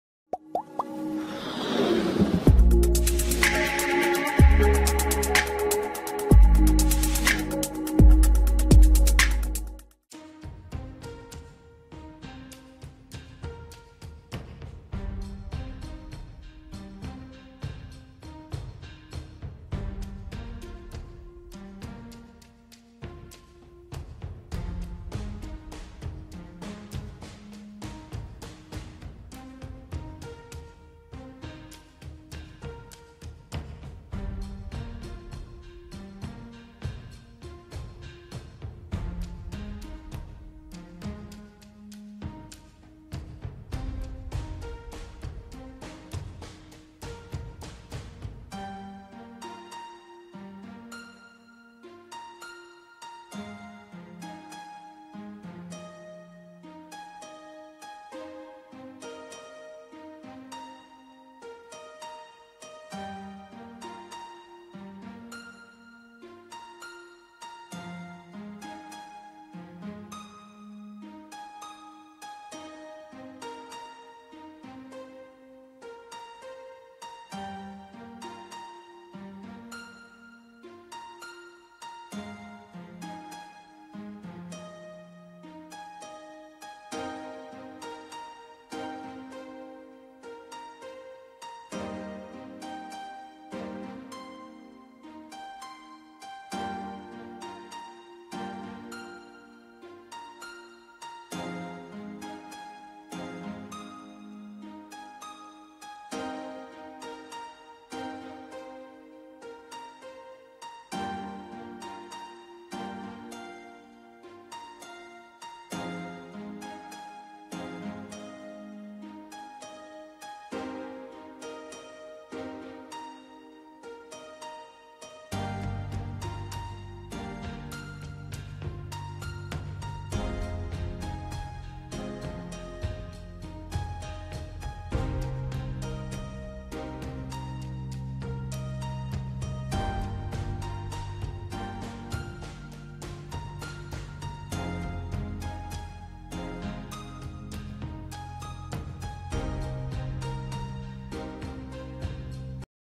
Una Vita, attrice di Genoveva Salmeron rompe il silenzio sul compagno: "Ecco come passeremo il Natal
Una Vita, attrice di Genoveva Salmeron rompe il silenzio sul compagno: "Ecco come passeremo il Natale" Sarà un Natale speciale per Clara Garrido.